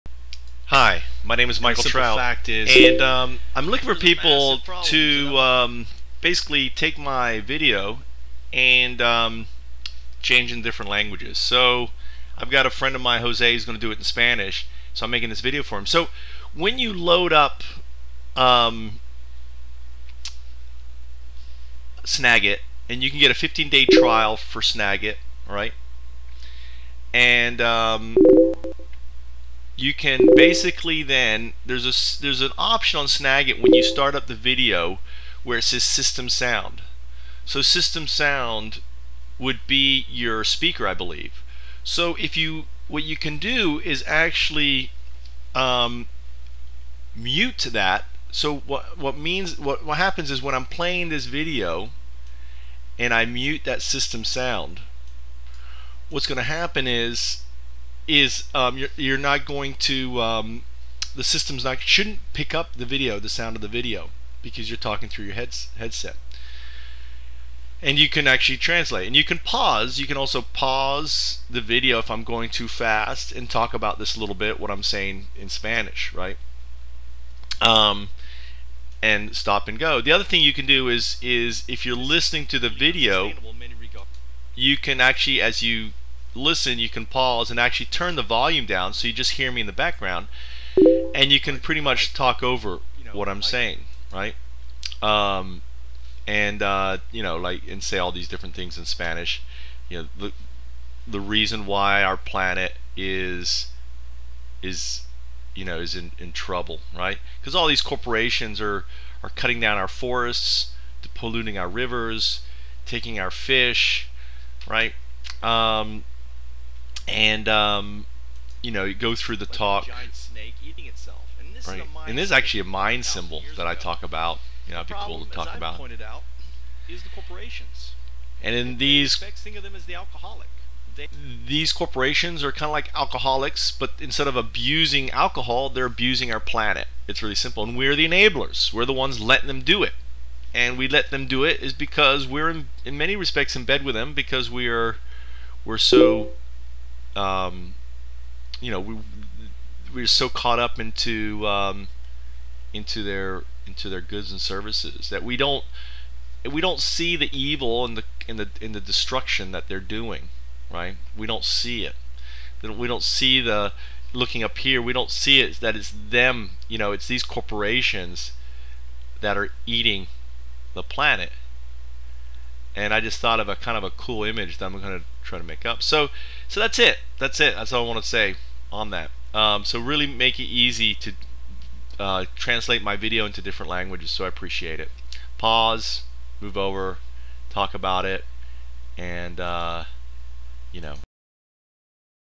How to translate a video into a different language
1, Get Snagit 15 day trial. They give you the full version.
2. Put my video in full screen and lover the volume to just loud enough you can here it
3. Mute system sound on the Snagit video controller. The button that shows up when you select what to screencast.
4. Listen pause speak or you can watch my presentation a few times talk along with it.